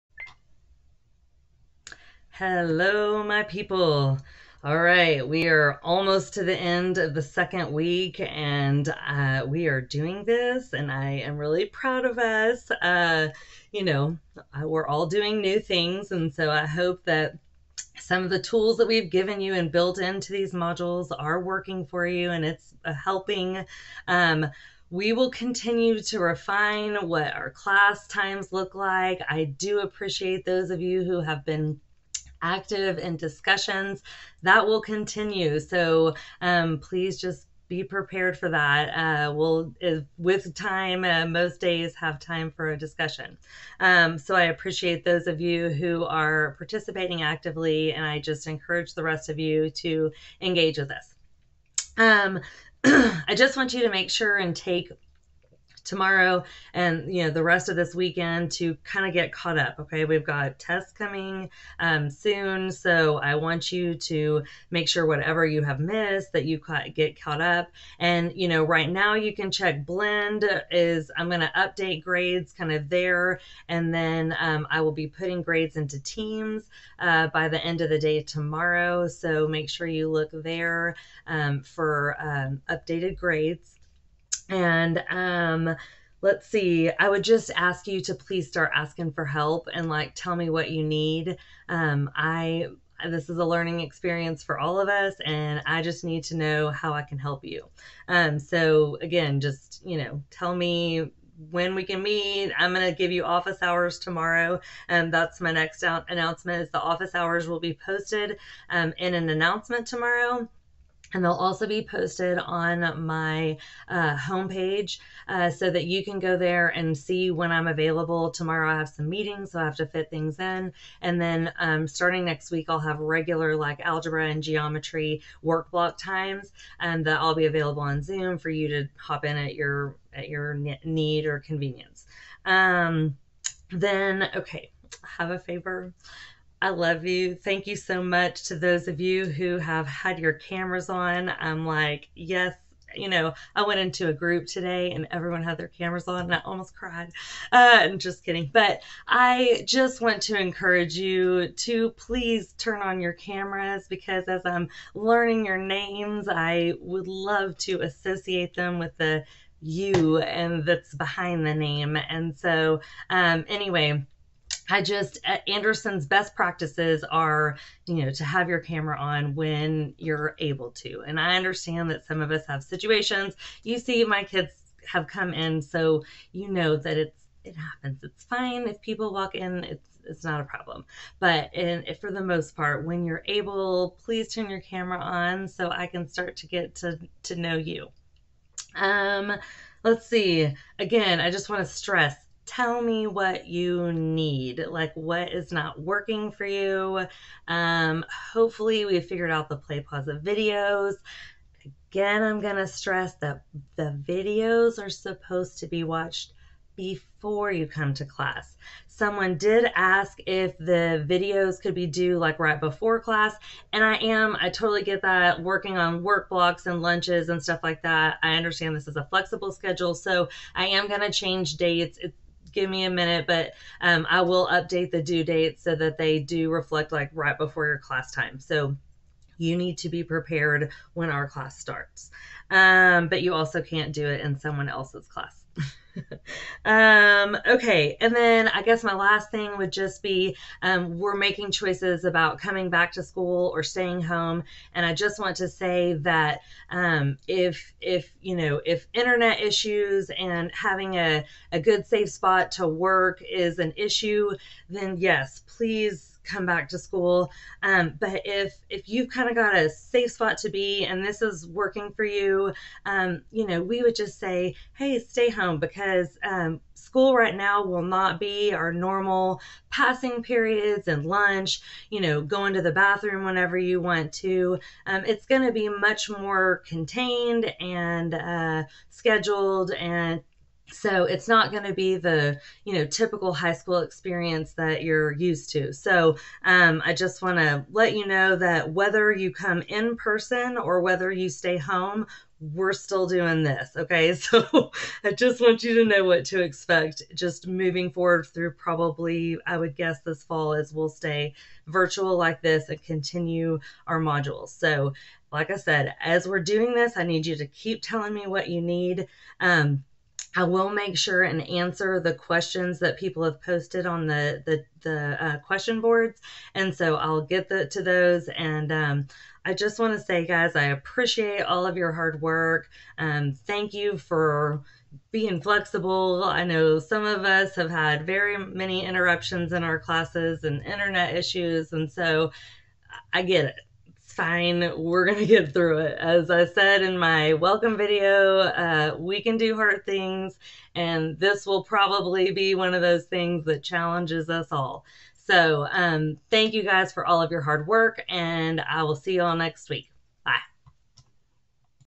Mrs. Lake's Week 2 Wrap Up
Some thoughts to share as we close our second week of school.